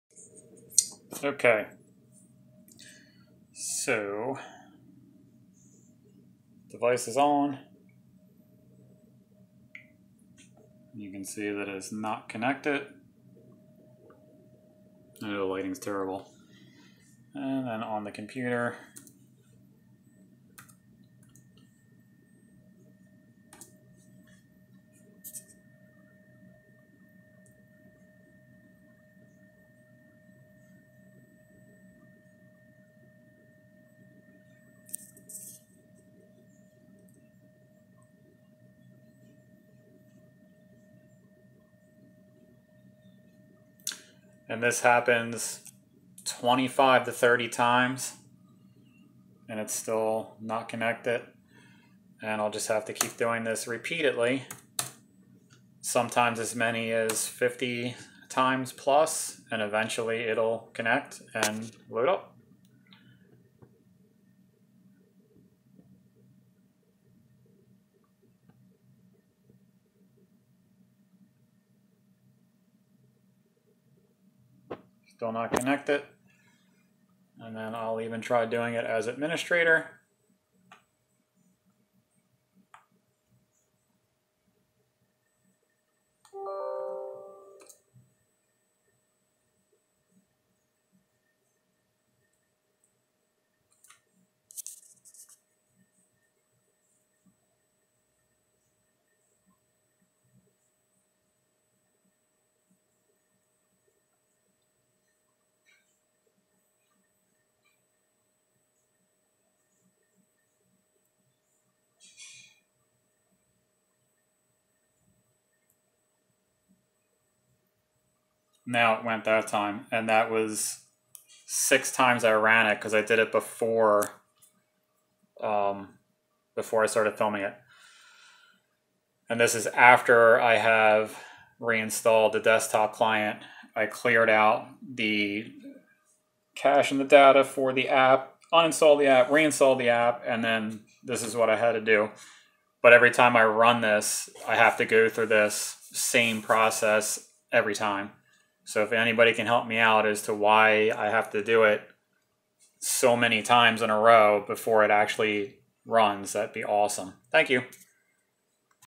Andomeda Not Connecting
Andromeda client for Substranum takes multiple attempts to connect to PC